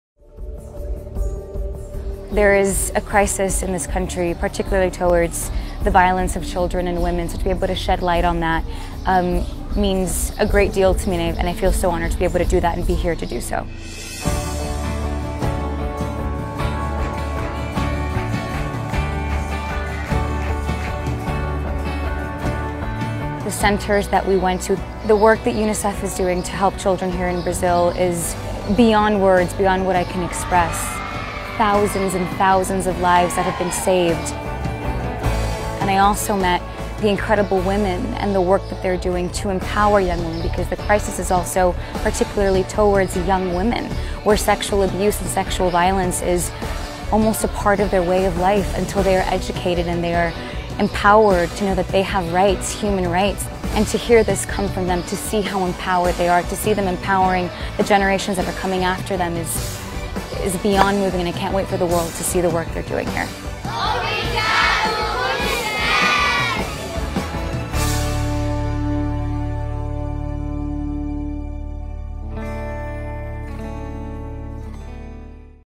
Atriz de filmes da Disney visita projetos do UNICEF em Pernambuco
A atriz e cantora norte-americana Sofia Carson esteve em Pernambuco para promover os direitos das crianças e adolescentes. A artista — que é apoiadora do Fundo das Nações Unidas para a Infância (UNICEF) — acompanhou atividades de empoderamento de meninas, educação e inclusão desenvolvidas pela agência da ONU e instituições parceiras no Recife e no semiárido pernambucano.

“É uma imensa honra estar no Brasil com o UNICEF para fazer o que eu puder para contribuir com esse trabalho tão inspirador e acabar com a violência contra as mulheres e as crianças”, disse a atriz, conhecida por sua atuação na franquia de filmes Descendentes, da Disney Channel, e na série Pretty Little Liars: The Perfectionists.